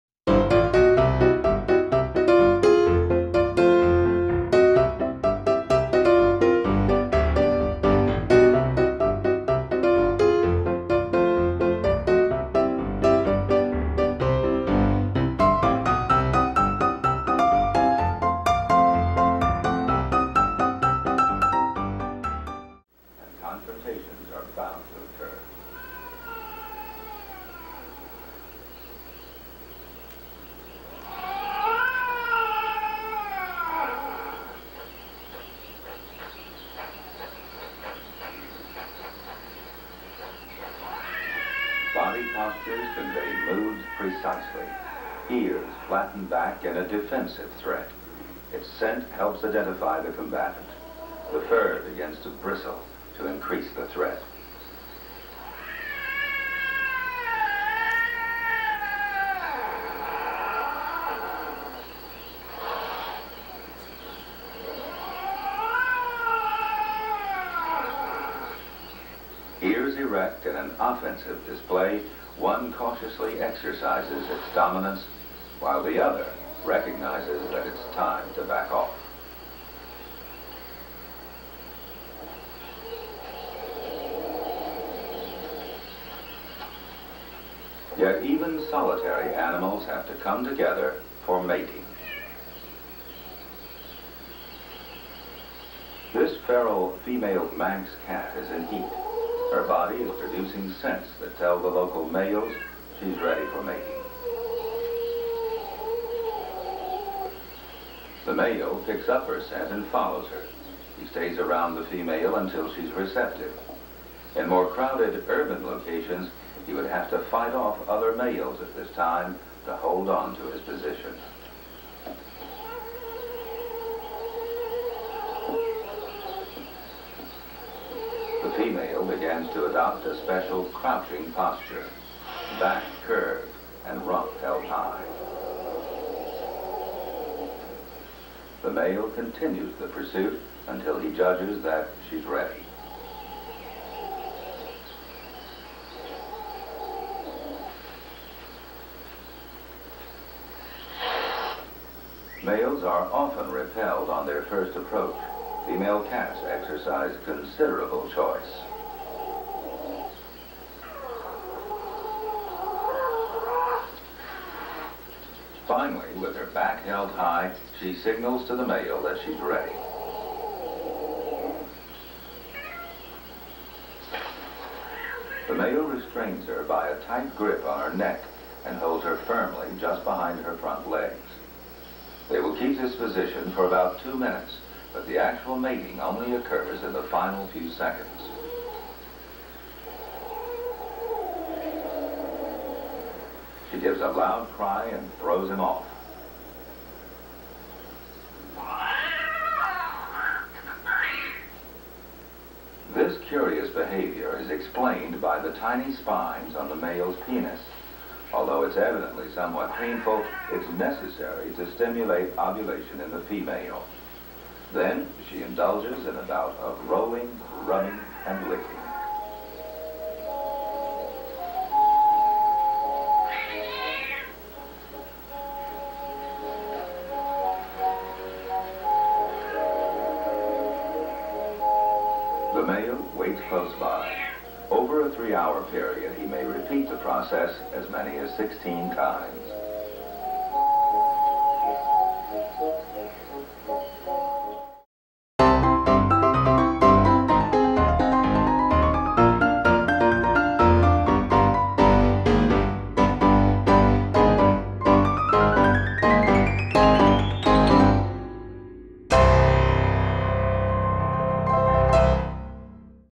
Freddie the Dog Watches TV
Freddie, a 13-year-old English Setter, watches a video about cats. He looks behind the TV to see where they went, but he can't find them. Freddie wonders, "How did they get inside the TV? Where are they hiding? Why can't I smell them?"